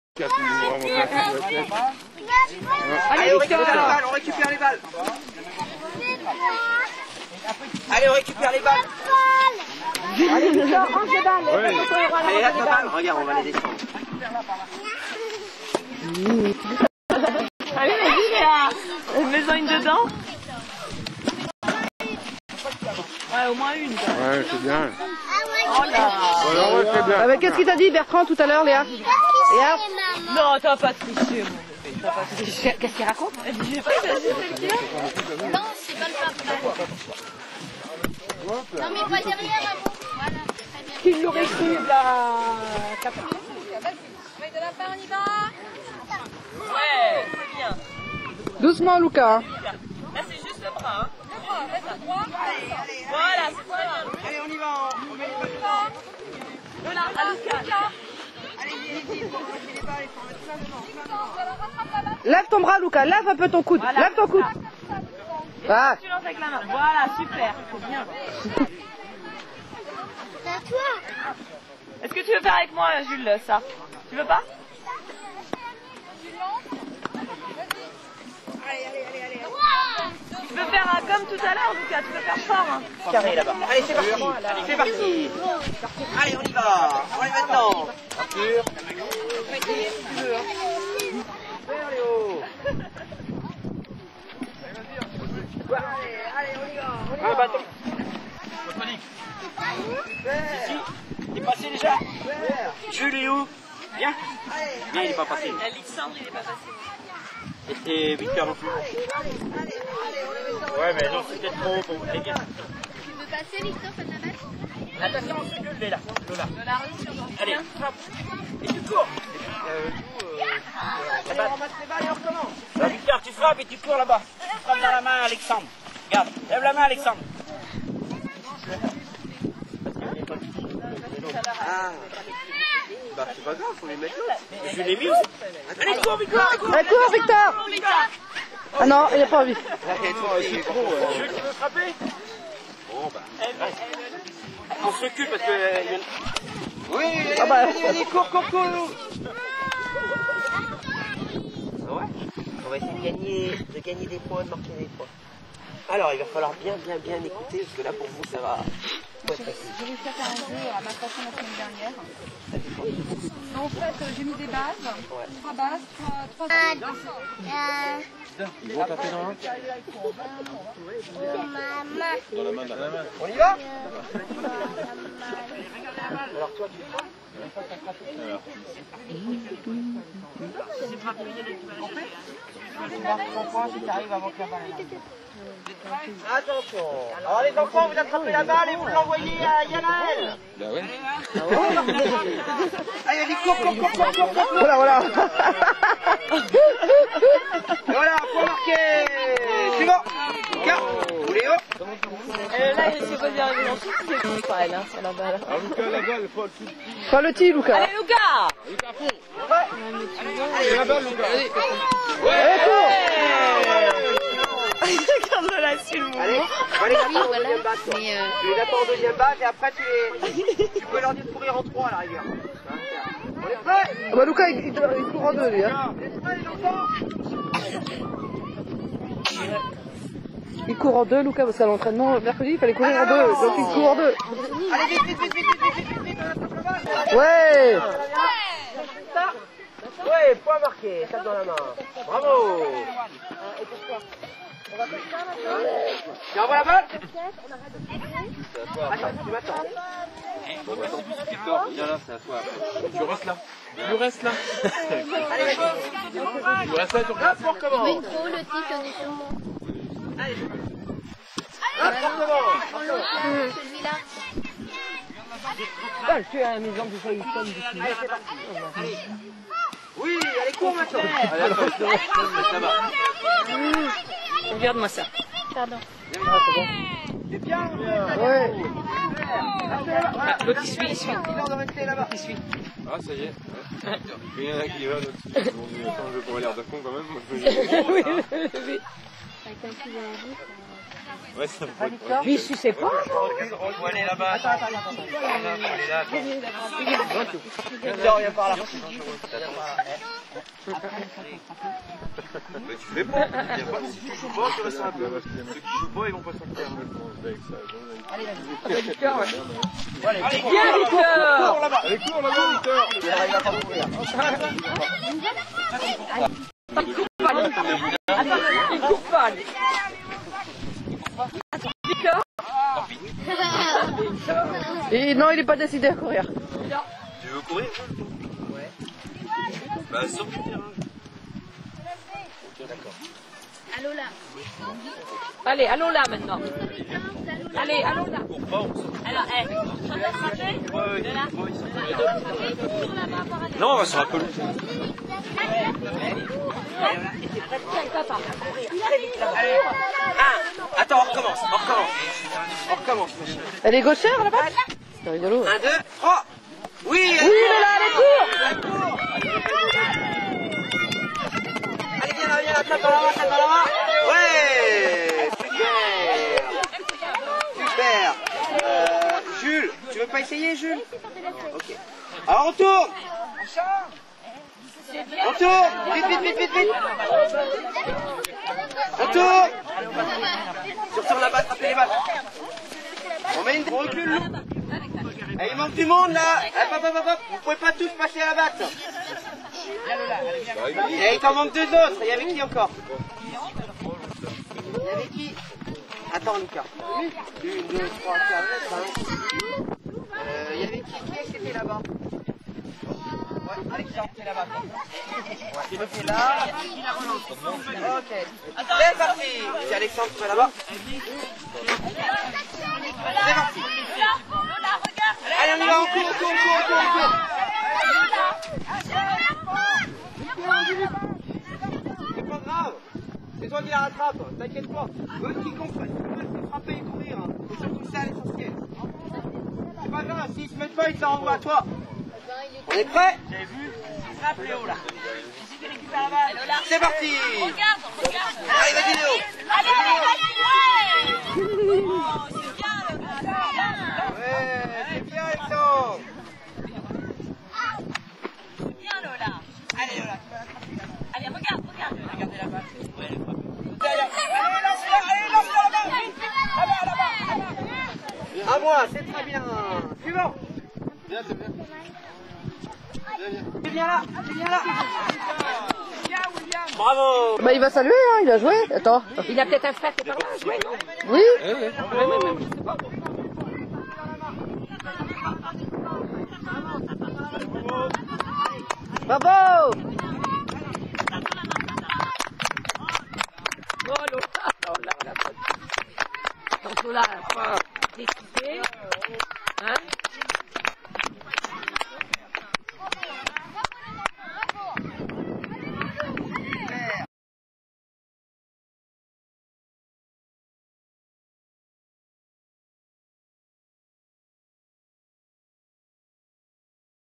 Baby Baseball chez les Black Panthers
Inititation au baseball des petits Bretons de Bréal sous Monfort et Maure de Bretagne.